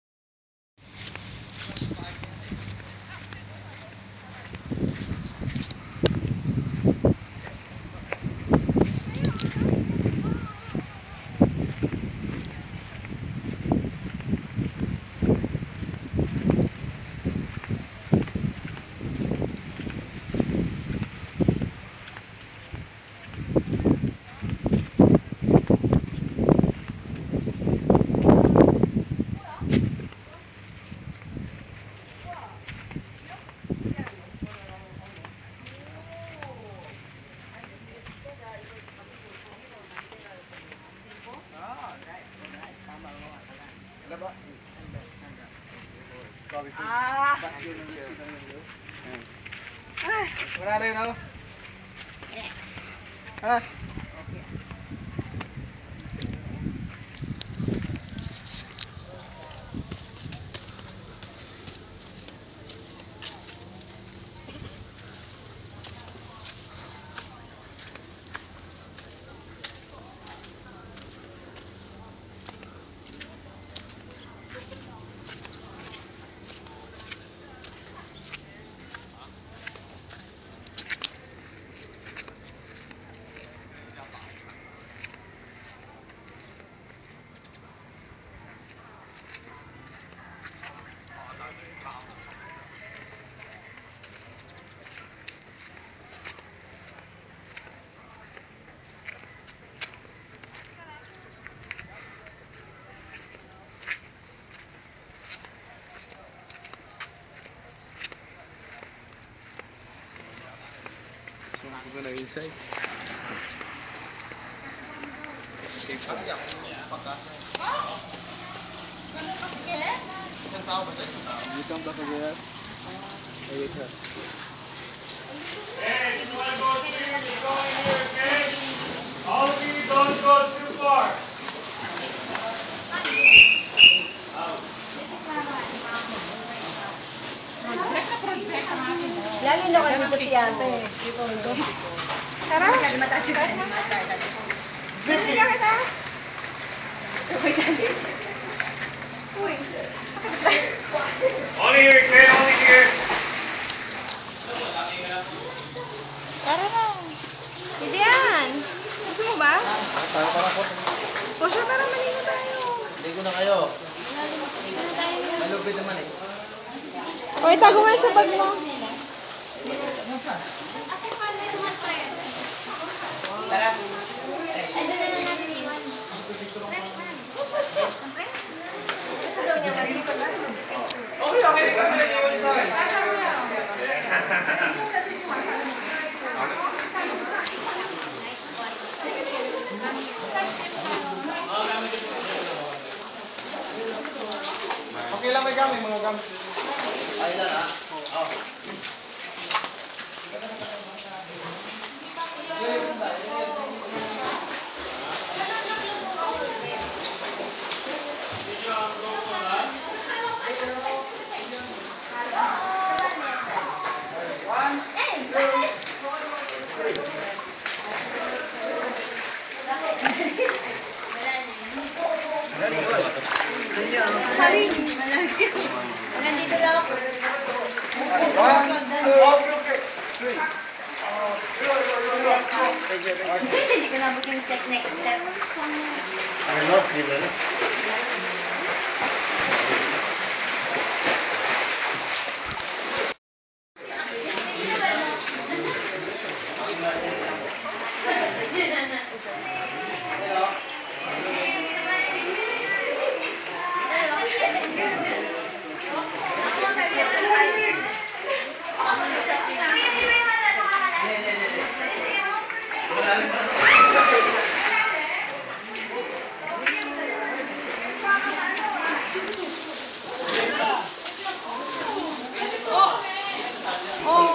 Crystal Cove Pt. 1
Crystal cove. a small island near boracay, Philippines.

video just taken by me with my cell phone while walking around, not the best quality but still some great things to be seen.